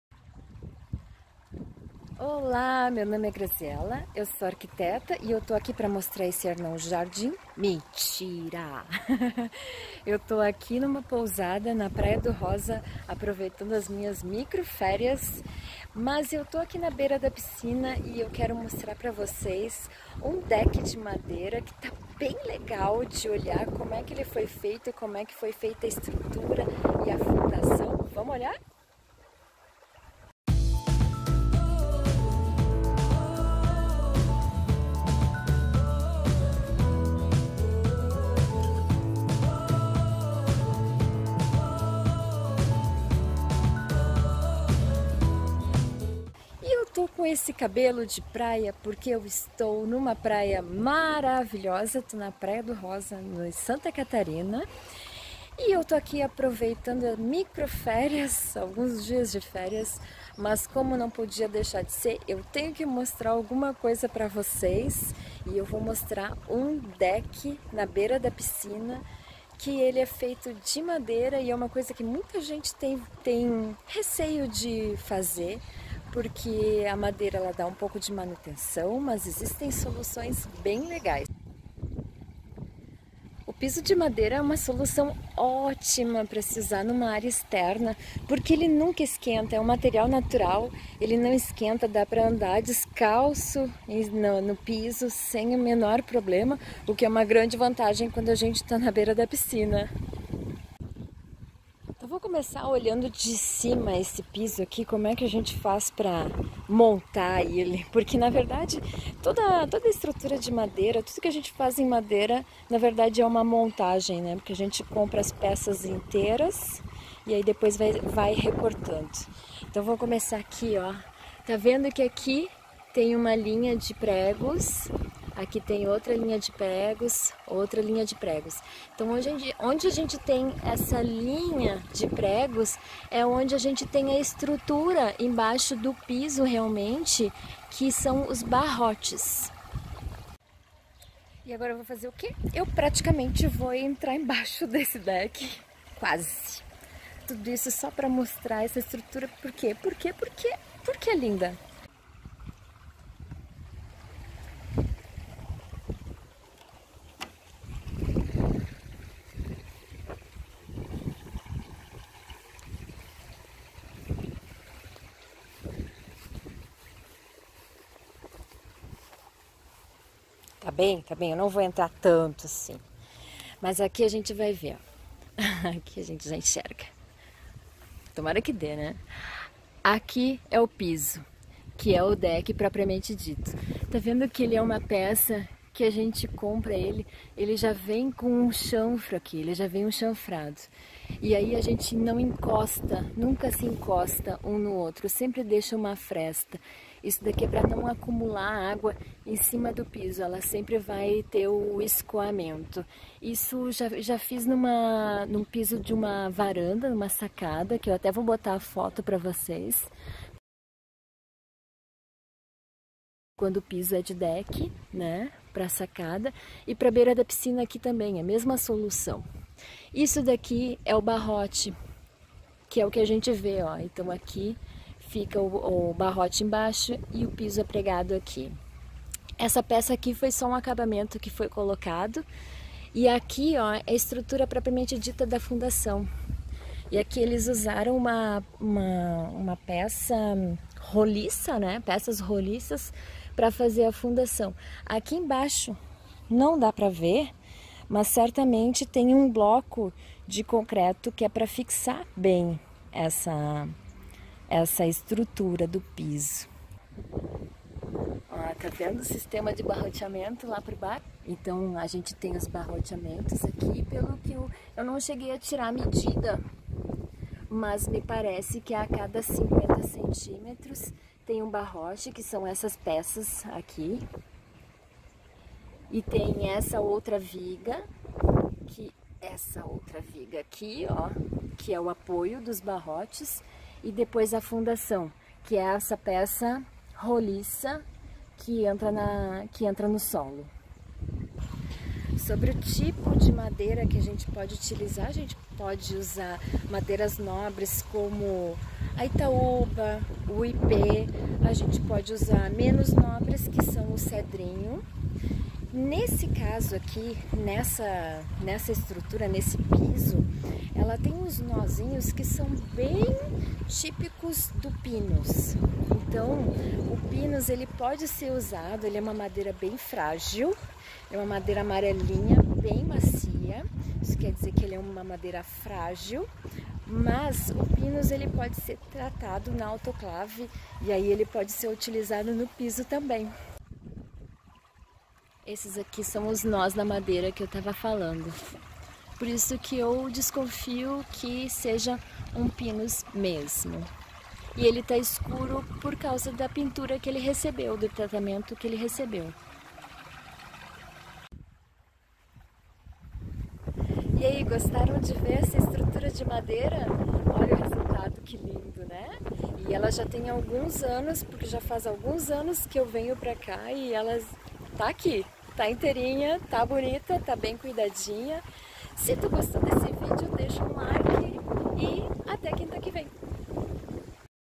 Entrei em baixo do deck de madeira da piscina: como é a estrutura
Entrei em baixo do deck de madeira da piscina. Estou de férias, na piscina, aproveitando o sol, mas não pude deixar de mostrar para vocês como é a estrutura de um deck de madeira!